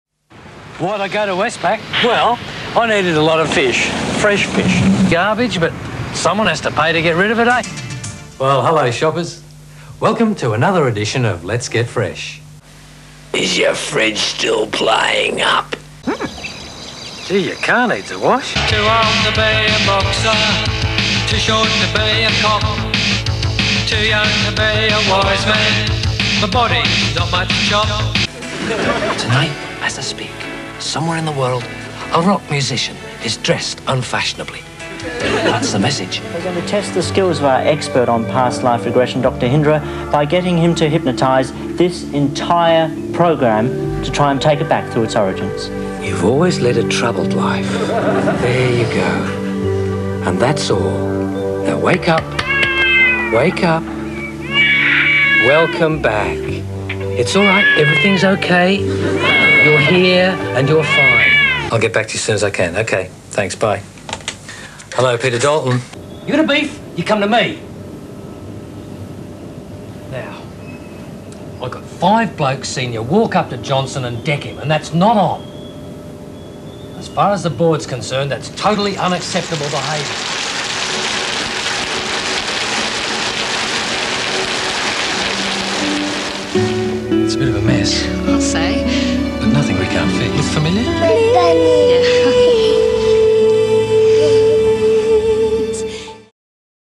My old showreel from when I thought I was an actor.
Some quick grabs from TV shows, Corporate videos, Training films, Short films, Commercials